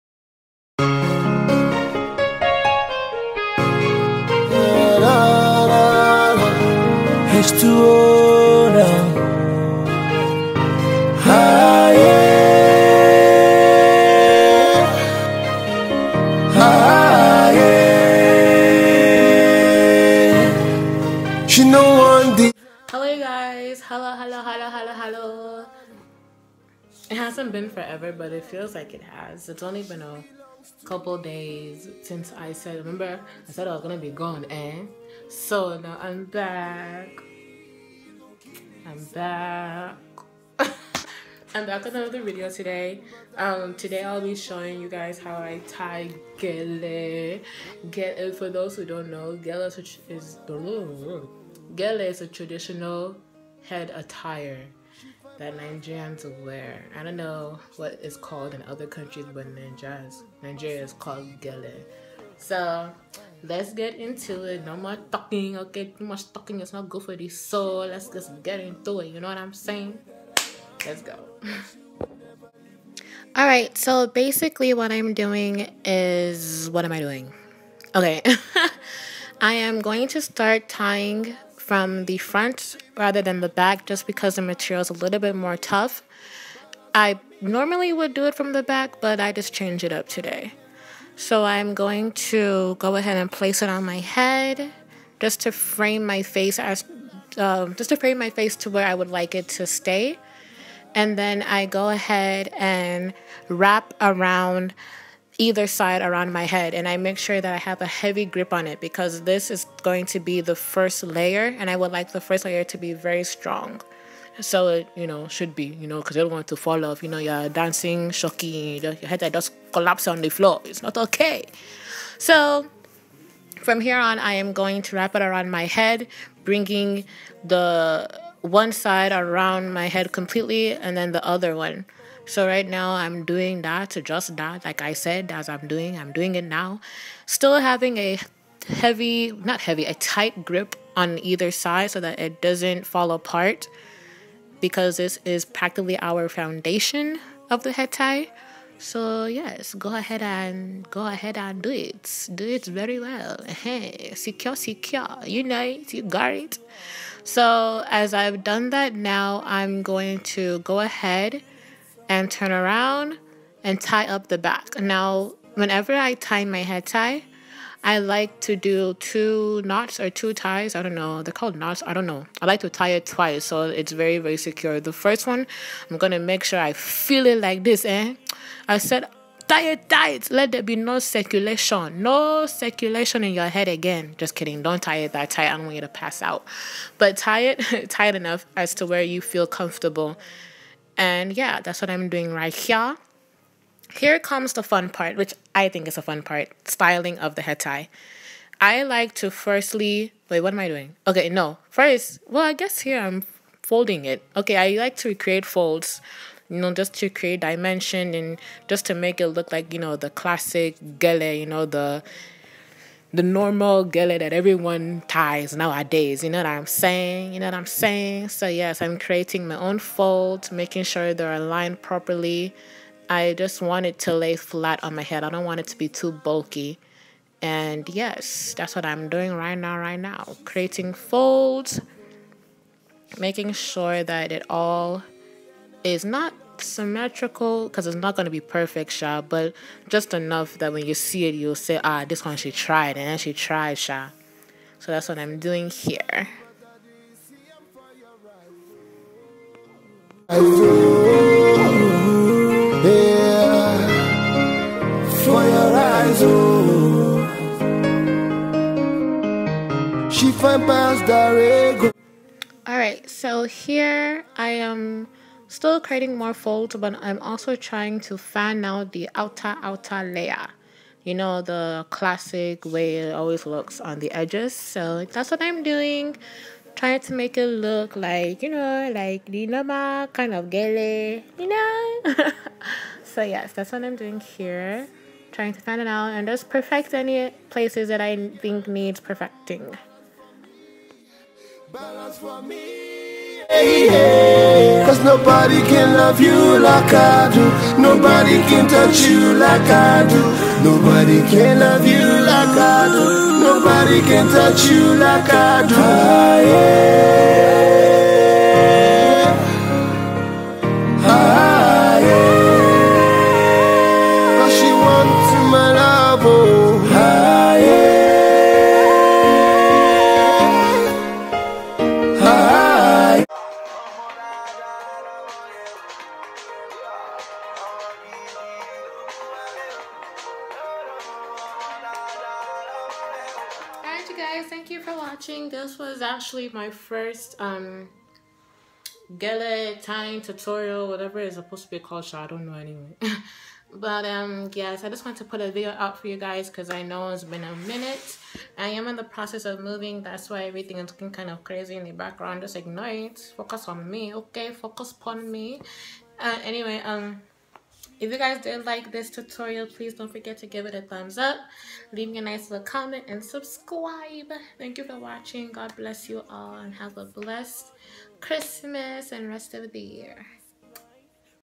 How to tie Aso Eke | Nigerian❤️
Don't forget to let me know the kinds of videos you'd all like to see. Xoxo.
Yo I didn't realize that my gele is Christmas colors aha. ❤️❤️❤️ Watch in HD ❤️❤️❤️